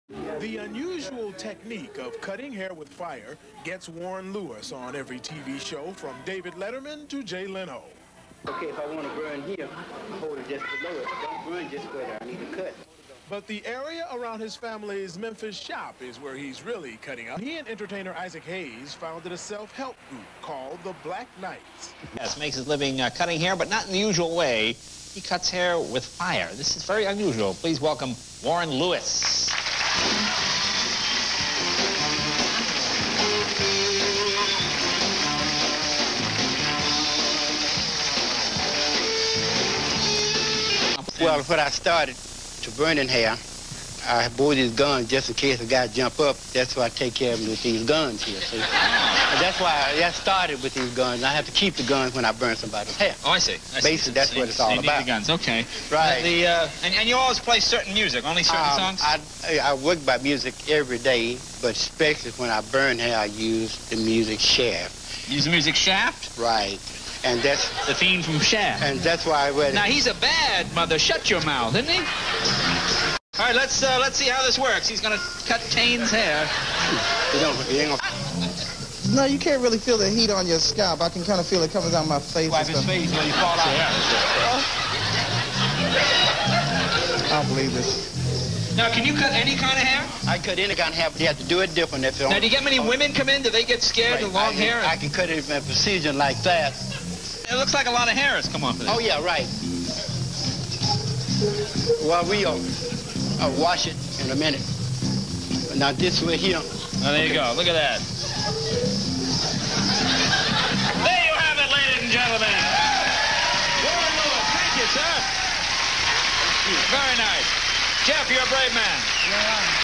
Warren Lewis-Fire Torch Barber
Warren Lewis resides in Memphis Tennessee and he cuts hair with Fire flames